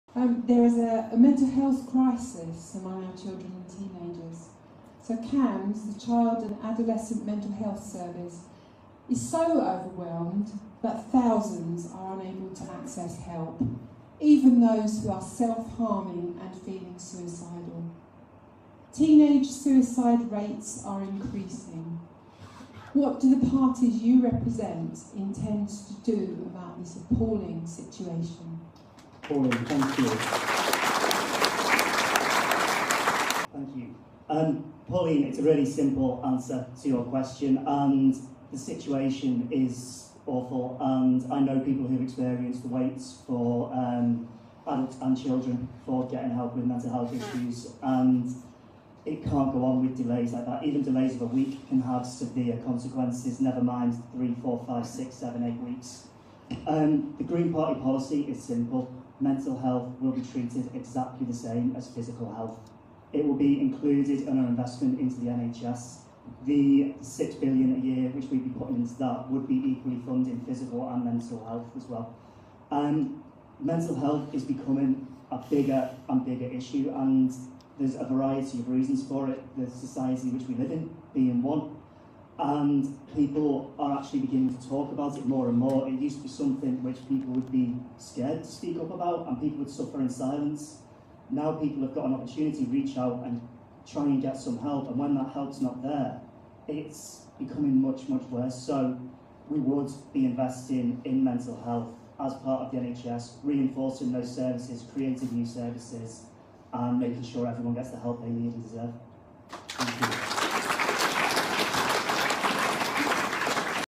Shortages of mental health provision can't go on: Chris Rigby, GE19 Christchurch hustings
Chris Rigby is the Green Party candidate for Christchurch in the 2019 General Election.

The hustings event at Twynham School on 26th November 2019, organised by Christchurch churches, also featured the other candidates: Christopher Chope (Conservative incumbent), Andrew Dunne (Labour), and Mike Cox (Lib Dem).